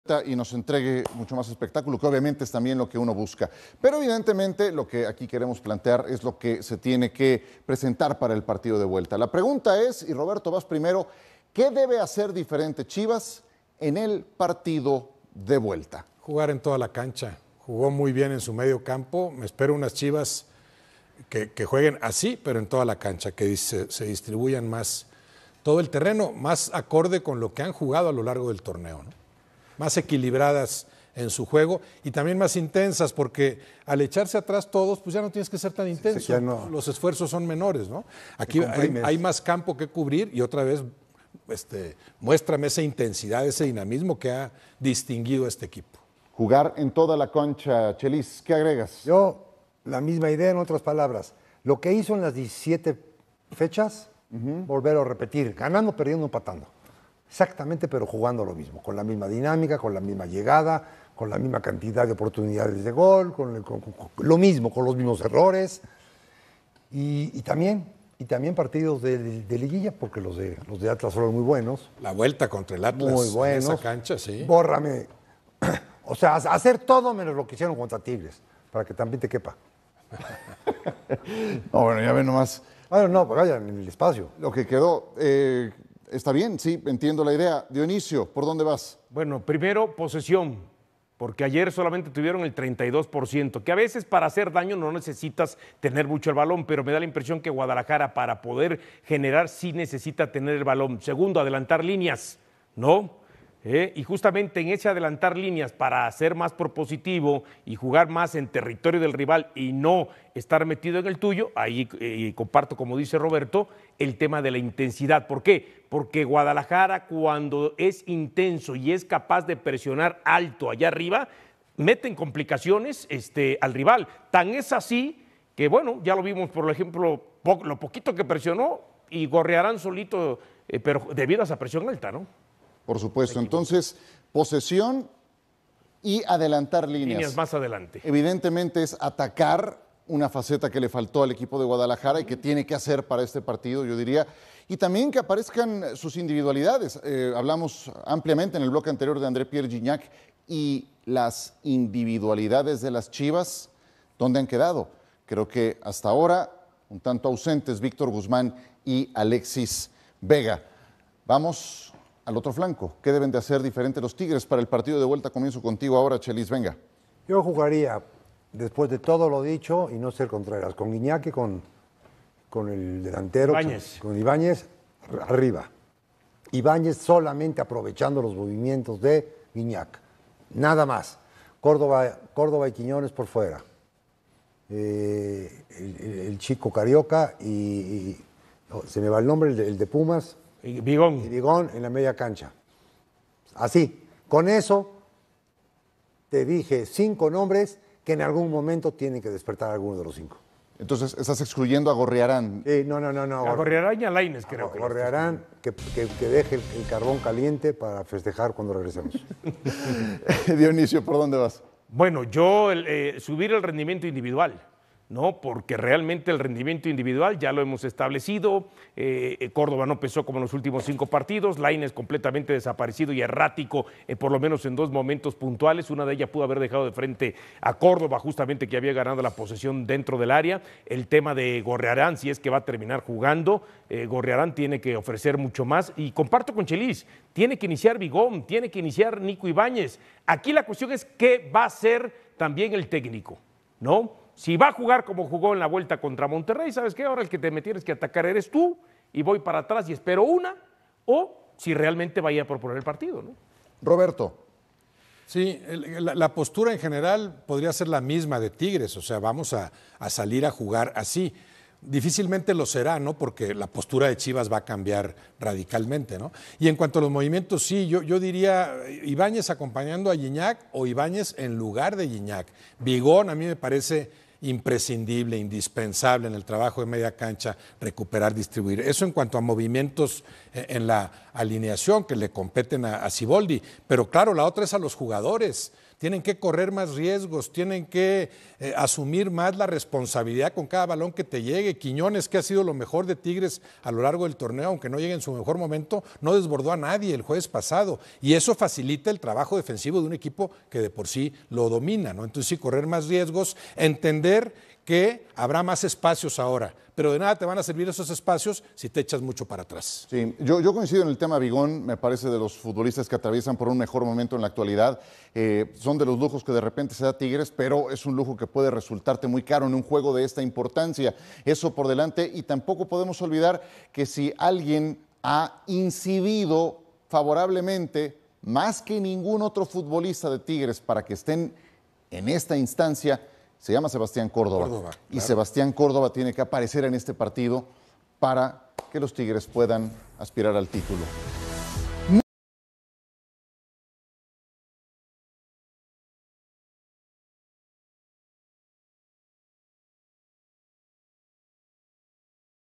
¿QUÉ CAMBIOS DEBEN HACER CHIVAS Y TIGRES PARA EL PARTIDO DE VUELTA DE LA FINAL?
Los analistas dan una opinión sobre qué cambios deberían hacer Chiva y Tigres para el partido de vuelta de la Final